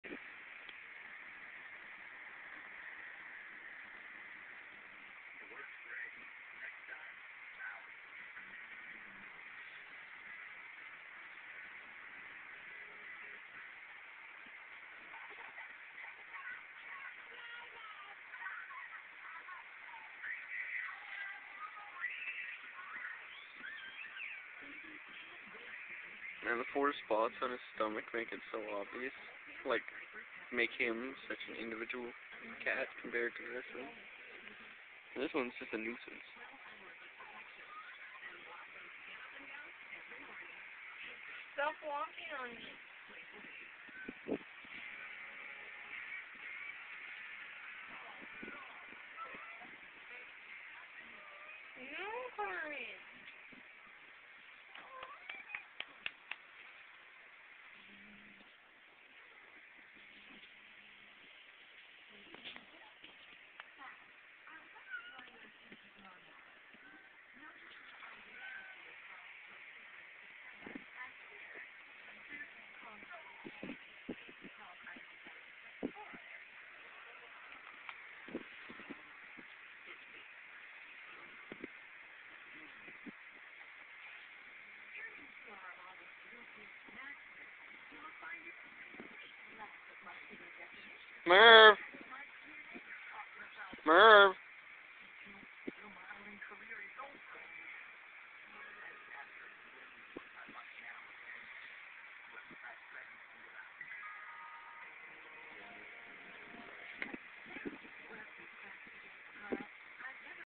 Cute kitten playing with other cat
Our new kitten, Spot (aka Spoticus the Spartan), is playing and gets beaten up by our cat Mervin! :)